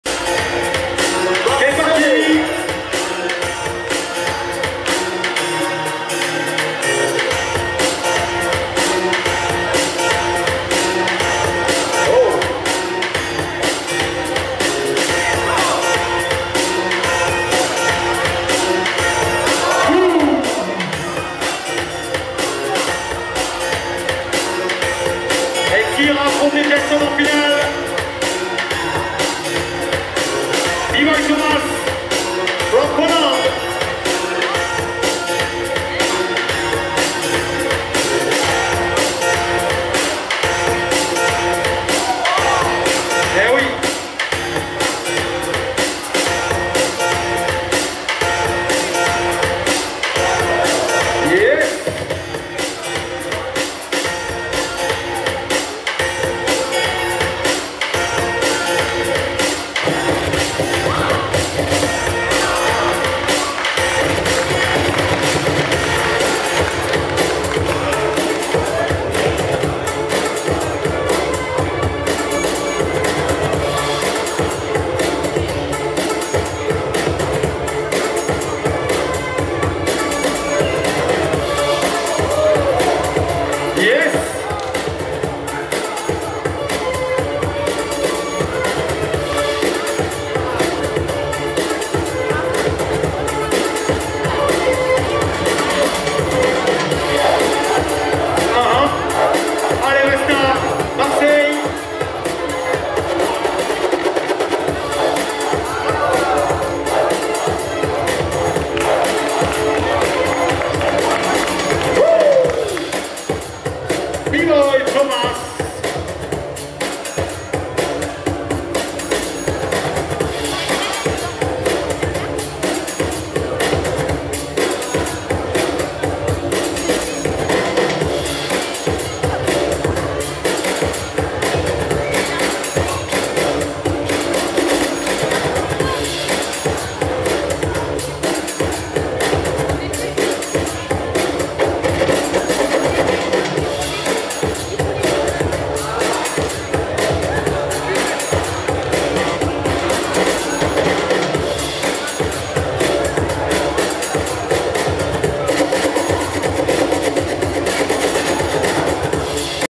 BATTLE AUTHENTIK 6 -DEMIFINALE 1v1- WESNA VS THOMAZ
Winner Thomaz
Dj Keysong